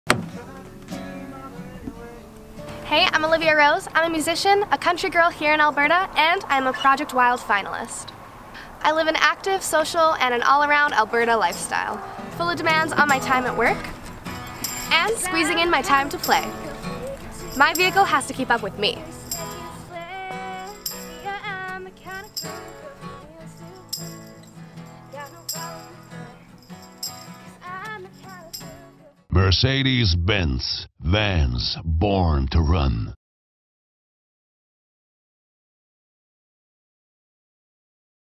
Mercedes-Benz Vans - Built to Handle Your Alberta Lifestyle | Olivia Rose - Project Wild Finalist
Project Wild Finalist, Olivia Rose is an Alberta local, musician, and a country girl with an active social and an all around Alberta lifestyle.

Whether it's her time at work or squeezing in time to play, her vehicle has to keep up. Mercedes-Benz Vans does just that.

Mercedes-Benz Edmonton West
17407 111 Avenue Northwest
Edmonton, AB
T5S 0A1
Canada